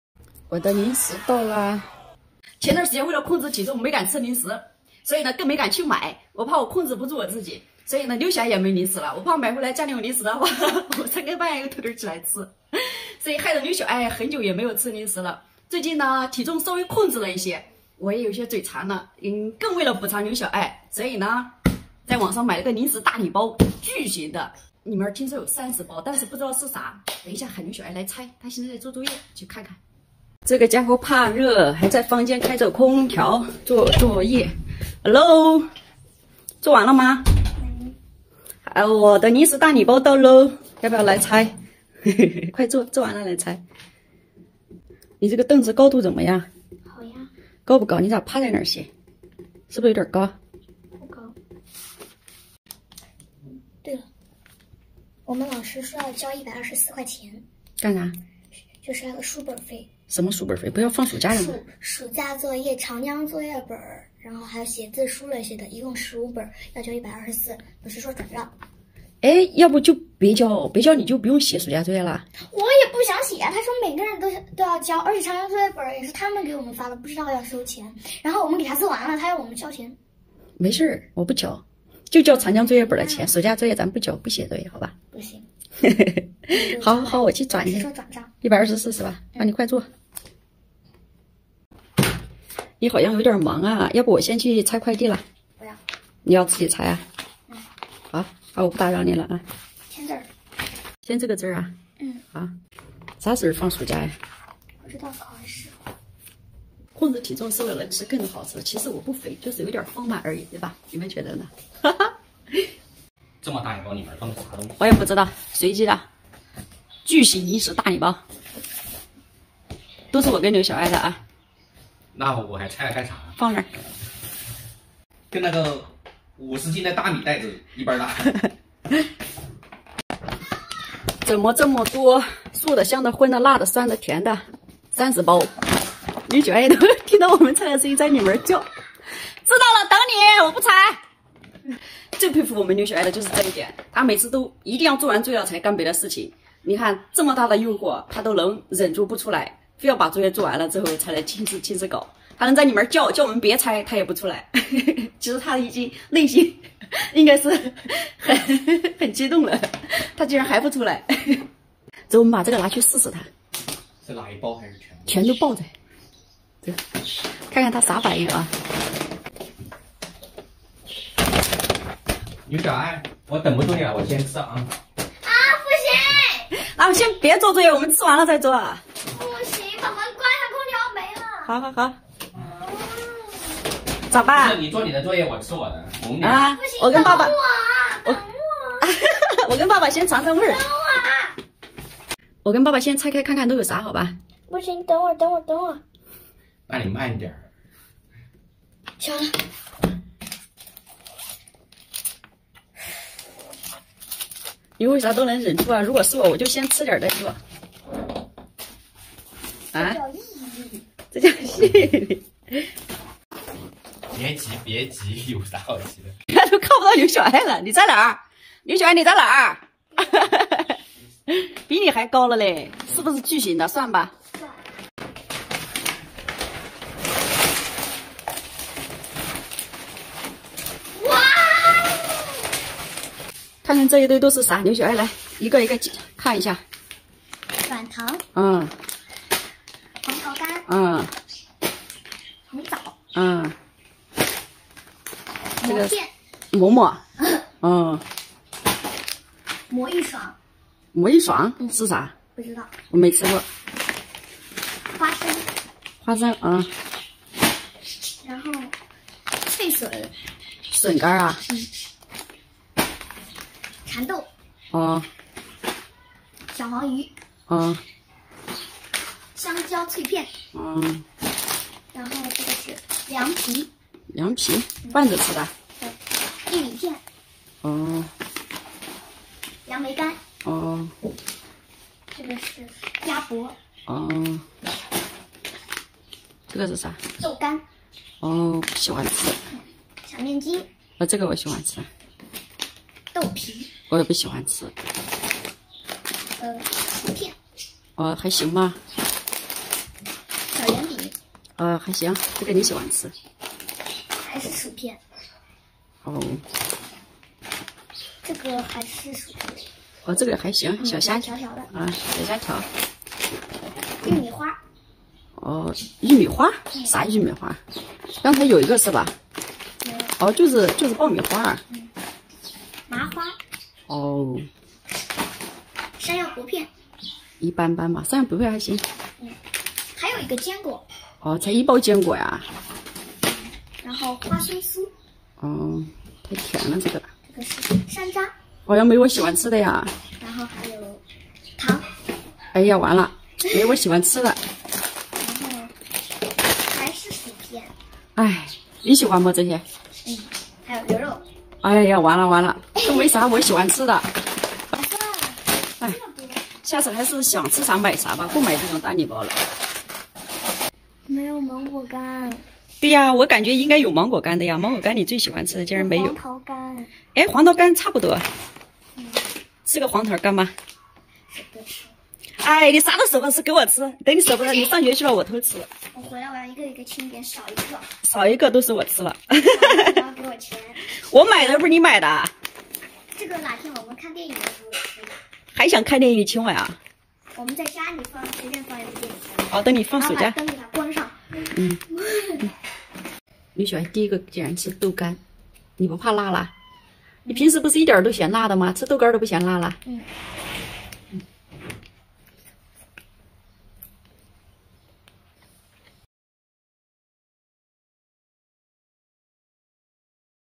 妈妈买一个巨型零食大礼包，小艾竟然能忍住不吃先做作业，佩服【吃饭睡觉逗艾艾】
小艾妈妈买了一个巨型零食大礼包，小艾竟然能忍住不拆，还先写完作业才吃，好有毅力~

我会这里记录并分享小艾成长路上的点点滴滴。